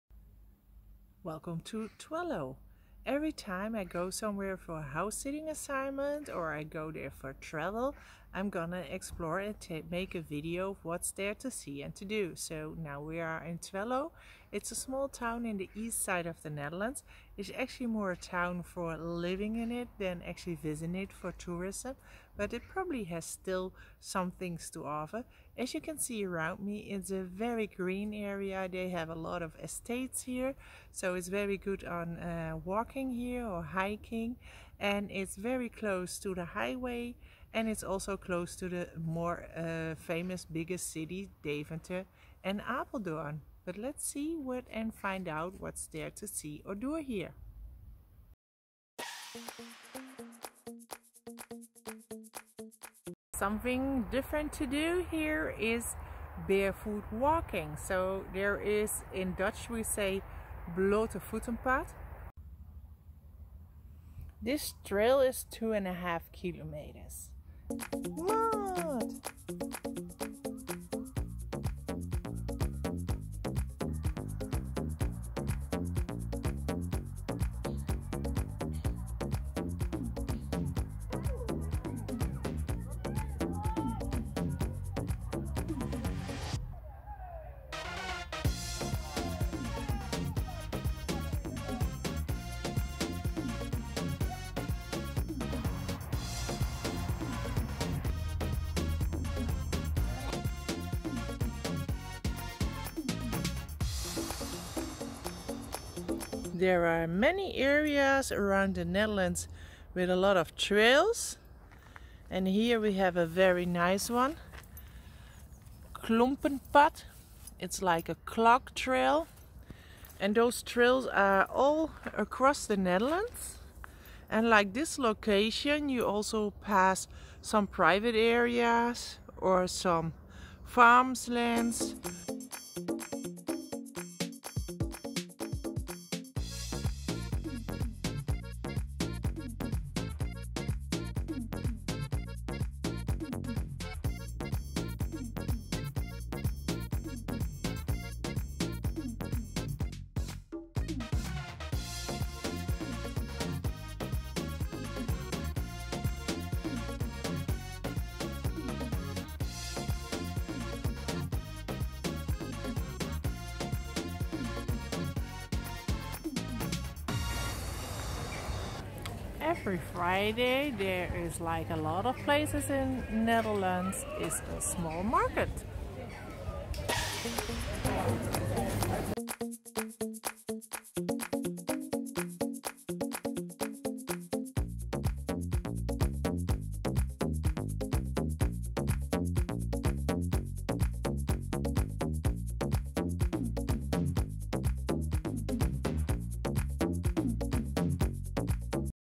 What is there to do in Twello?
Twello is a village with approximately 13,405 inhabitants in the municipality of Voorst, in the Dutch province of Gelderland. The village is located in the rural area between the cities of Deventer and Apeldoorn, close to the river IJssel.
Twello is more a village where people live than it has tourist attractions. It is a green area with many country houses.
At the edge of the village lies Hof van Twello. Beautiful to walk the barefoot path. 2.5 kilometers through mud, water, a bamboo field etc. Delicious and laugh for young and old. In the greenhouses and the fields are different things grown. In the accompanying shop you can buy various fresh and organic products.
In and around Twello are 6 clog toads. These hiking and biking trails mainly combine nature with a bit of culture. Many of the paths are about farmland or private land. As a result, it is not always allowed to take a dog with you or to keep it on a leash.
In the immediate vicinity is Teuge airport. Here you can get flying lessons and / or parachuting.
Also in the immediate vicinity is Bussloo. A large recreational lake. Of course you can swim there. However, there are also many other sports such as water sports but also waves or visit the wellness.
The center has several shops and restaurants. The local swimming pool has a beautiful indoor and outdoor pool. Every Friday afternoon is the market in the center.